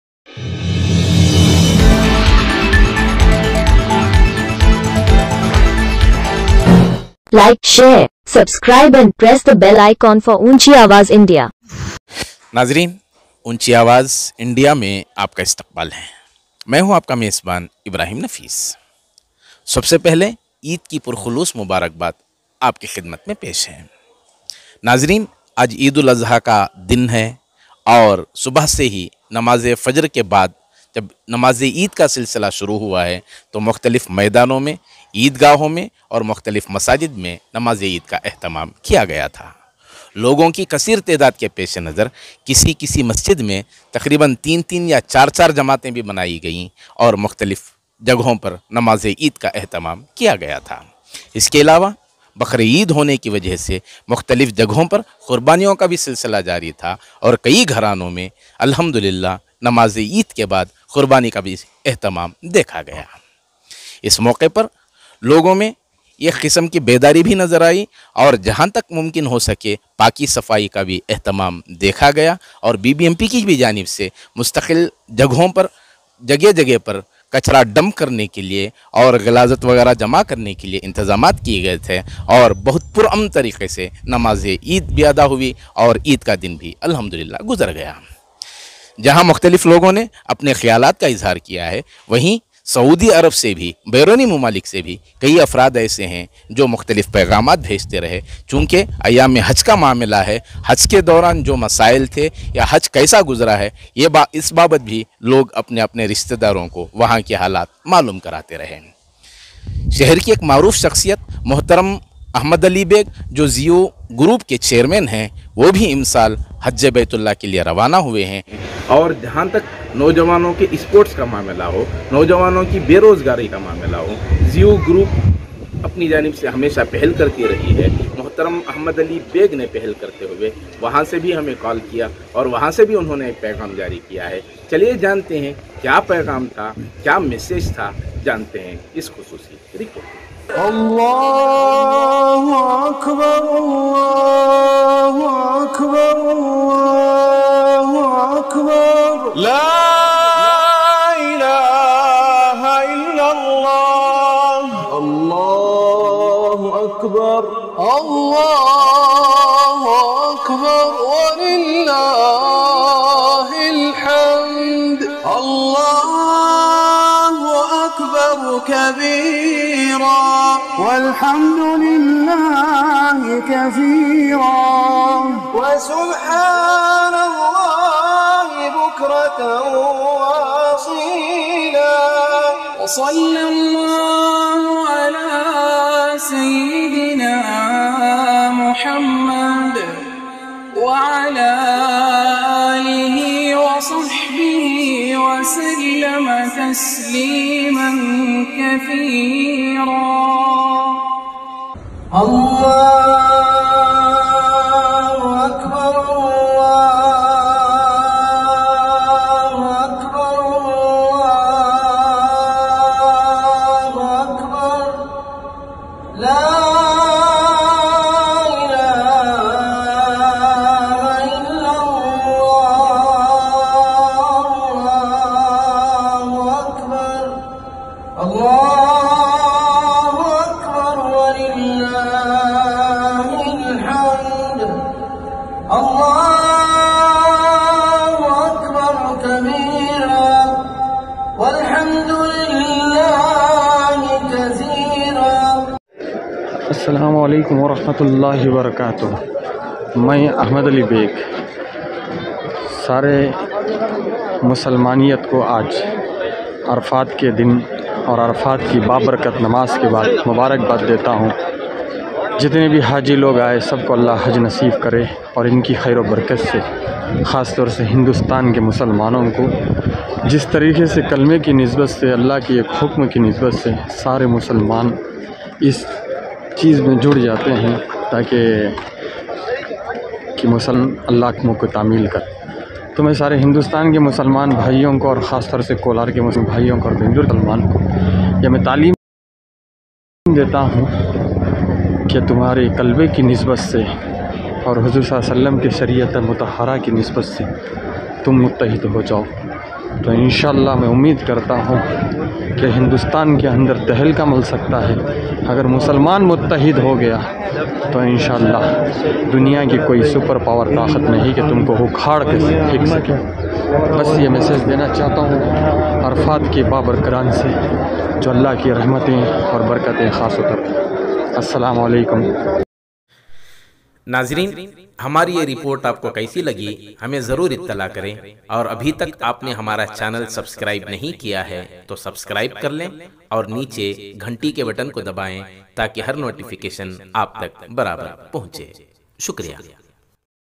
EID KA PAIGHAM KYA HAI || QURBANI ||ITTEHAD
EID UL AZHA KI MUBARAKBAAD ZIYU GROUP KE CHAIRMAN AHMED ALI BAIG NE MAIDAN E ARAFATH SE DIYA ITTEHAD KA PAIGHAM